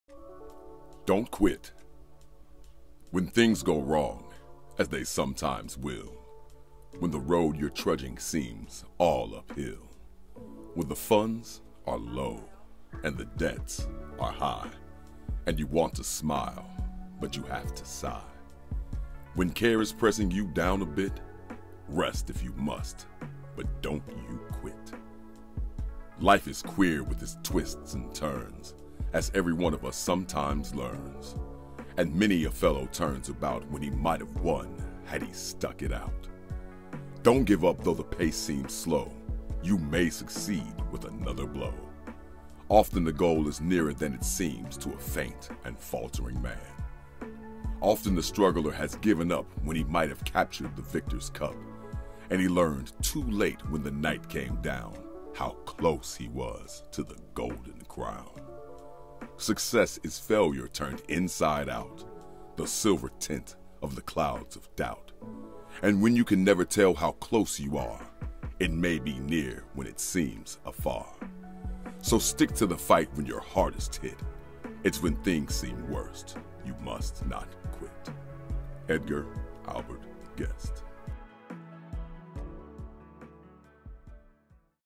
🎧 Don’t Quit by Edgar Albert Guest lofi chill Study Music poetry 🎧
Welcome to our serene corner of YouTube where timeless poetry meets modern chill beats. Today, we’re featuring “Don’t Quit” by Edgar Albert Guest, a motivational classic, accompanied by relaxing lofi music perfect for studying, relaxing, or unwinding.

🌟 Why You’ll Love This:

 • Inspiring and uplifting poetry
 • Smooth lofi beats for concentration and relaxation
 • Ideal background music for studying, reading, or chilling

📜 About the Poem:
Edgar Albert Guest’s “Don’t Quit” is a powerful reminder to keep pushing forward, no matter how tough life gets. It’s a poem that has motivated many to stay resilient and hopeful in the face of challenges.

🎶 Lofi Beats:
Our custom lofi beat complements the poem’s message, providing a calming and focused atmosphere. Perfect for background music during study sessions or a peaceful evening at home.

Thank you for tuning in and remember, no matter how tough it gets, Don’t Quit. 💪✨